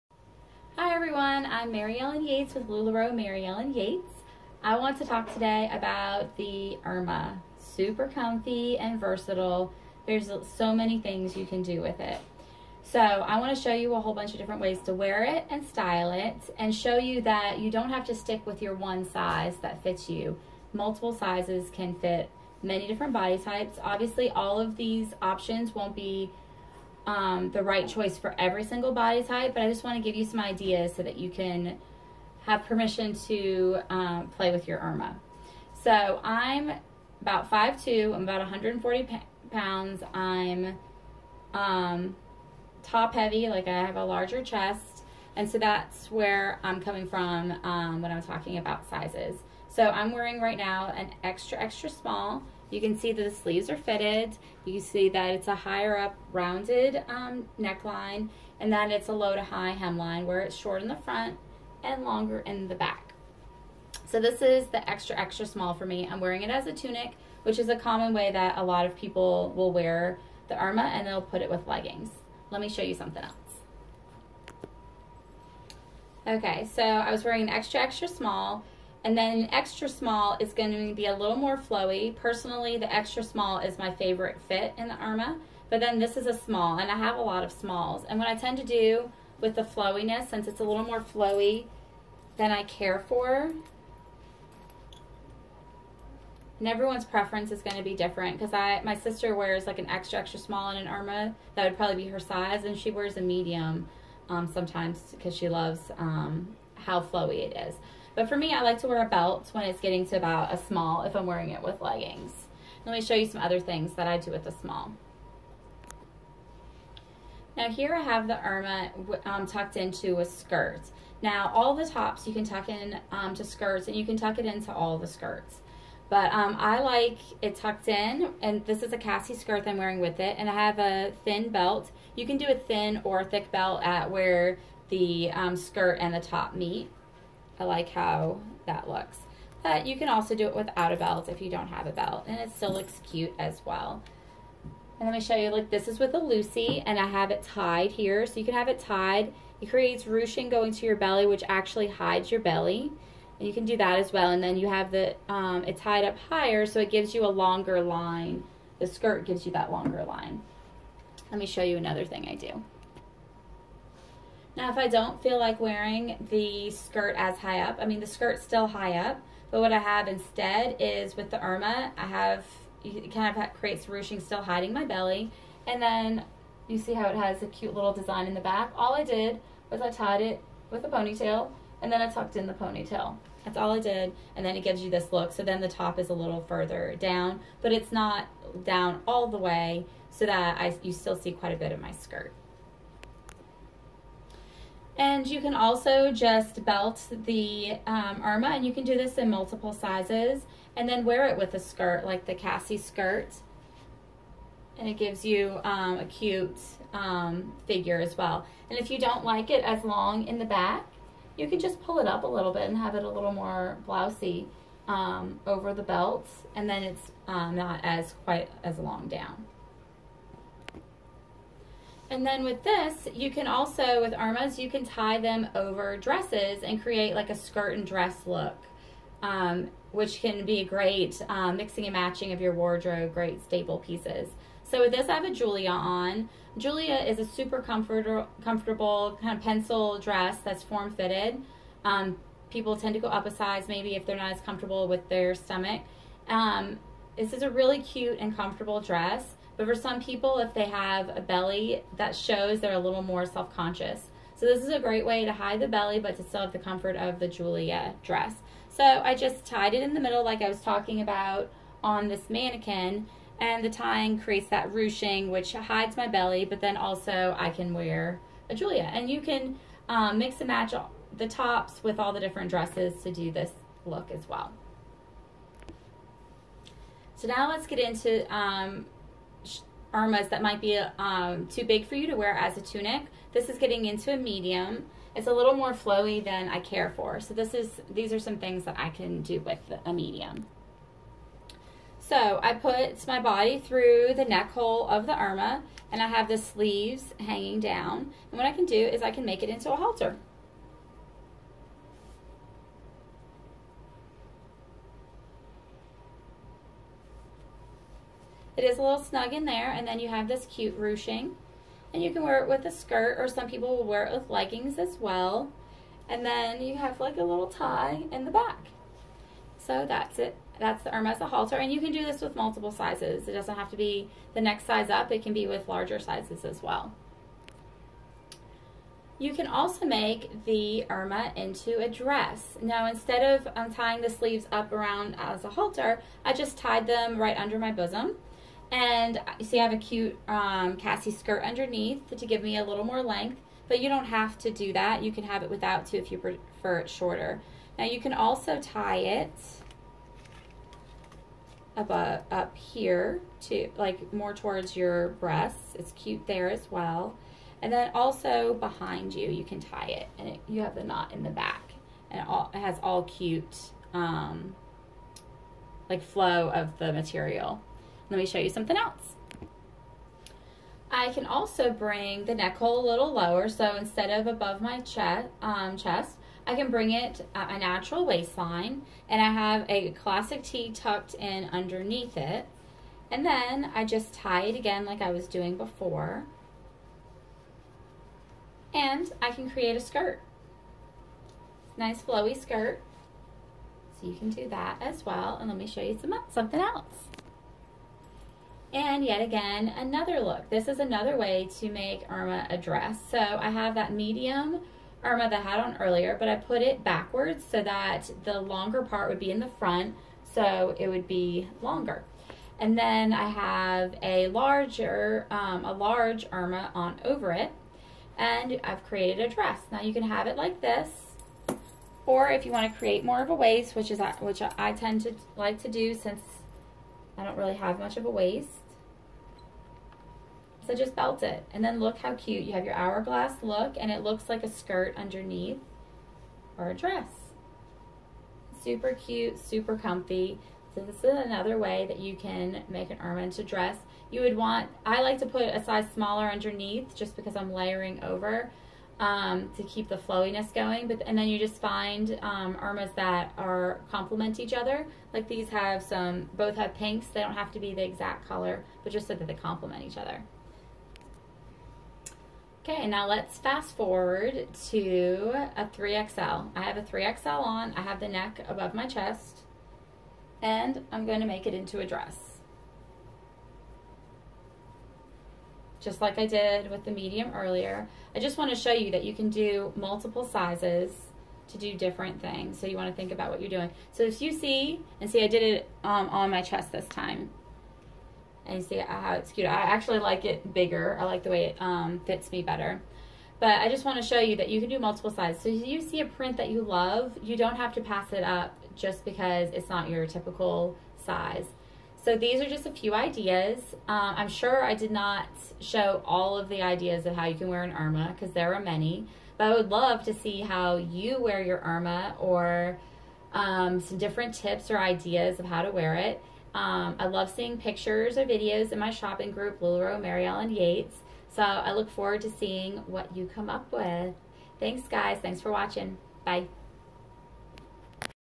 How to Style & Wear the LuLaRoe Irma in Multiple Sizes
The LuLaRoe Irma is a comfortable and versatile piece that should be a staple for every wardrobe. Learn how to wear and style it in multiple sizes and as various pieces. Did you know the Irma can be worn as a tunic, skirt, dress, halter top and much more? Find out how!